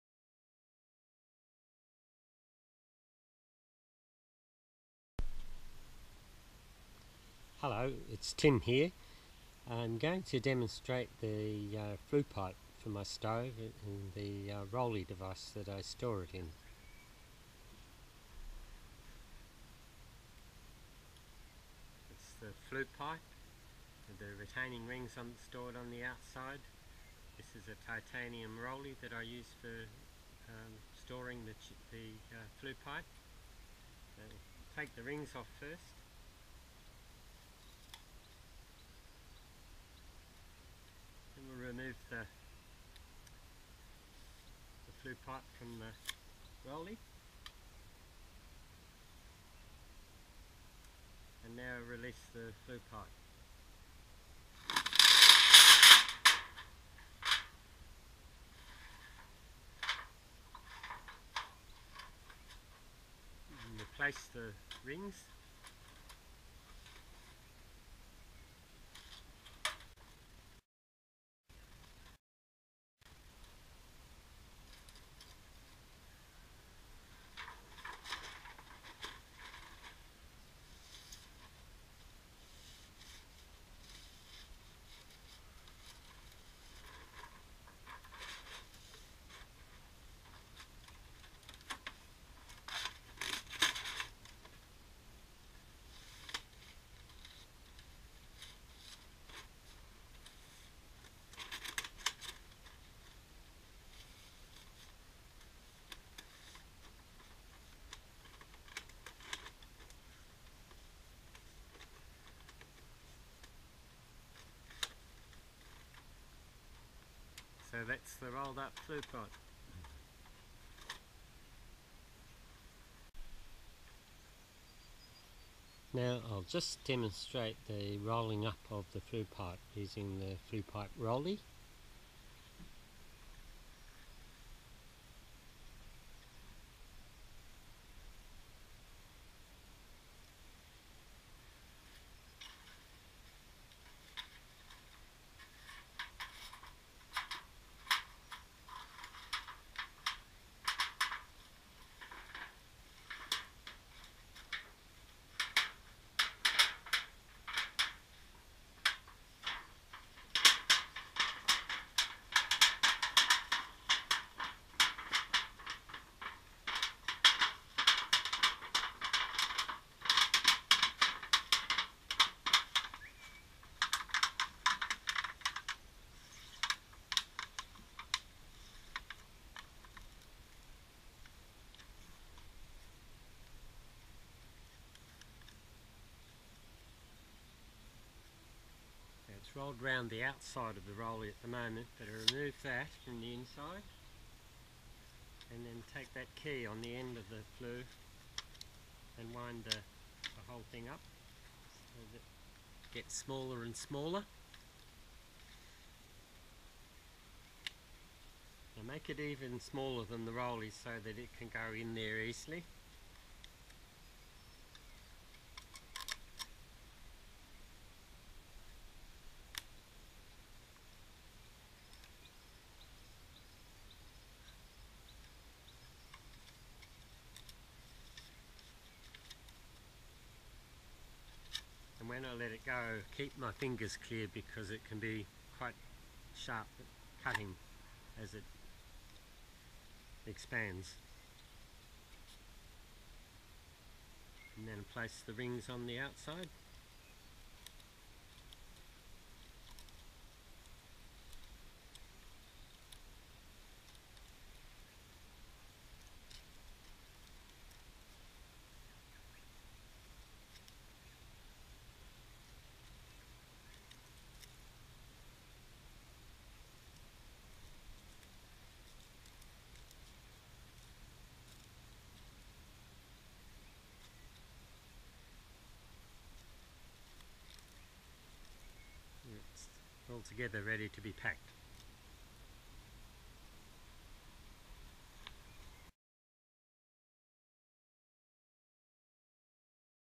Tent stove pipe assembly and pack up
This is a video of the assembly of my roll up flue pipe for my tent stove. It also includes the rolling of the pipe and the packing of the pipe using my rolley storage tube. Sorry for the audio quality (left the Gopro outer case on). see my DIY camera mount to address this problem